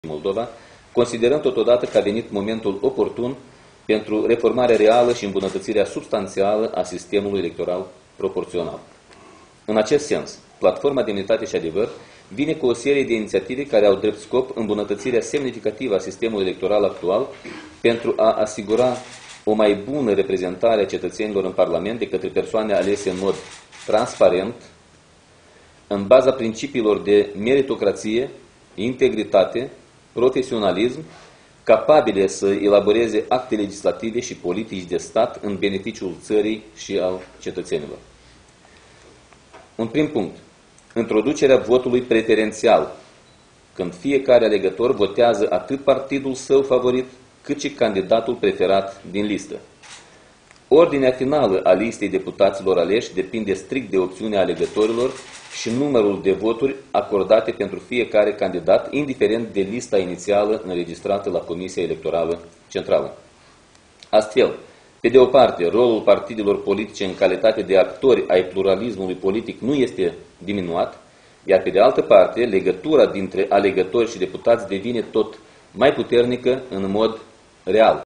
Conferință de presă cu tema „Propunerile Platformei Demnitate și Adevăr cu privi...
Titlul evenimentului: Conferință de presă cu tema „Propunerile Platformei Demnitate și Adevăr cu privire la &#238;mbunătățirea sistemului electoral proporțional &#238;n beneficiul cetățenilor”